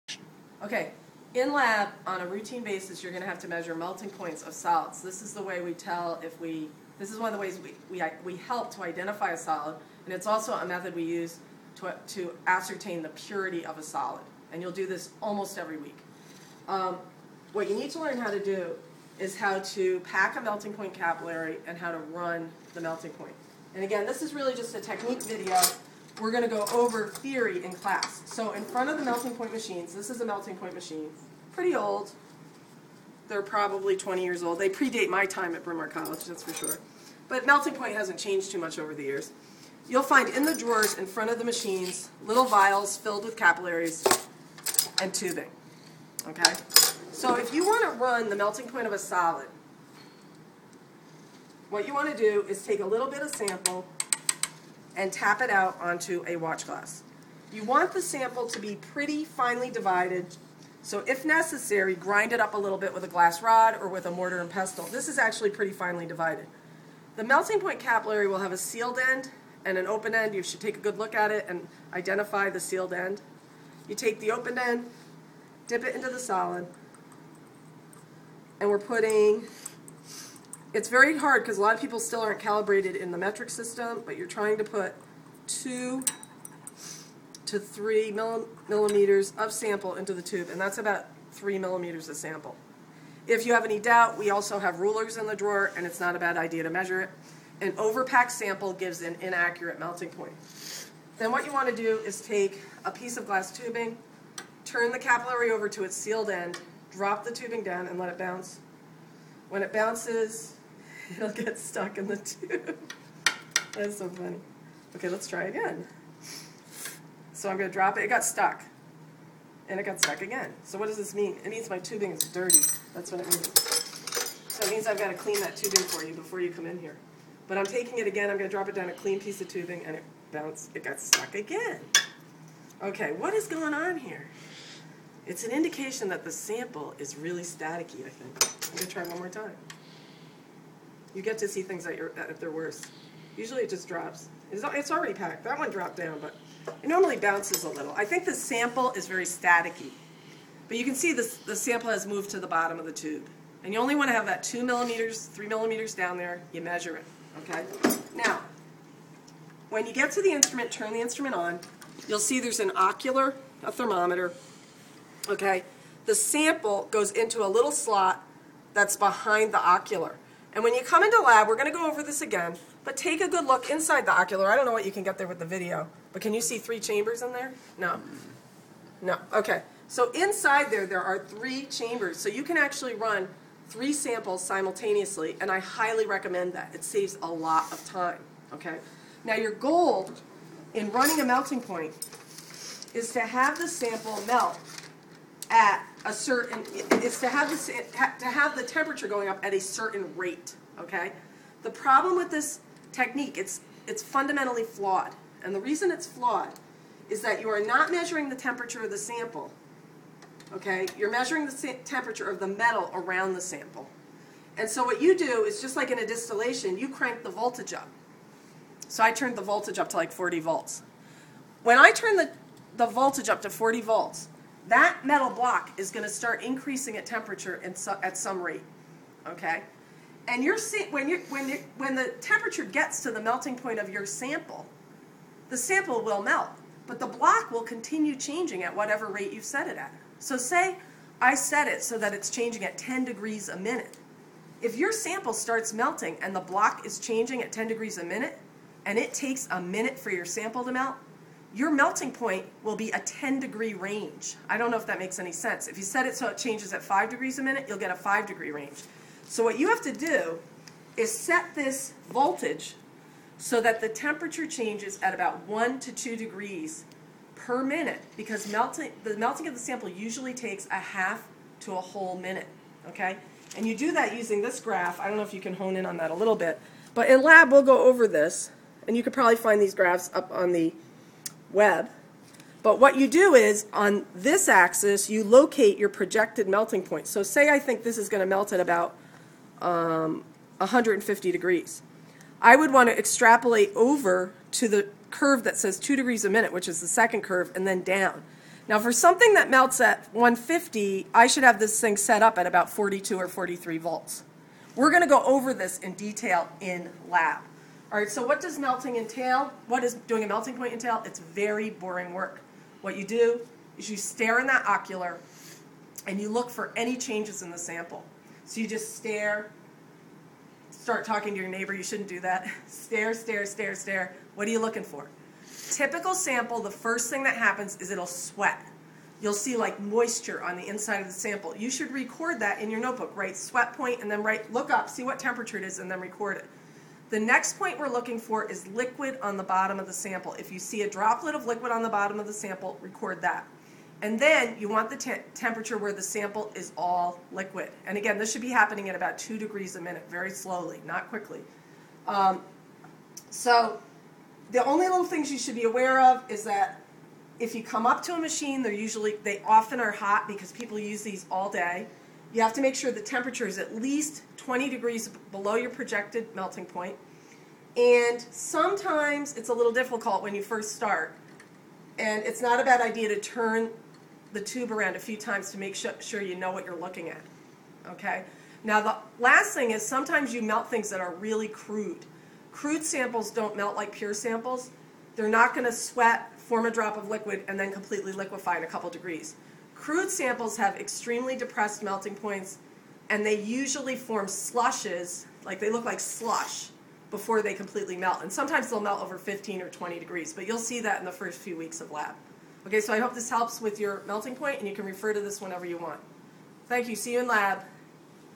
How to pack a melting point capillary and measure a melting point
Dr. Nerz at Bryn Mawr shows the basics of how to pack a melting point capillary and how to measure a melting point.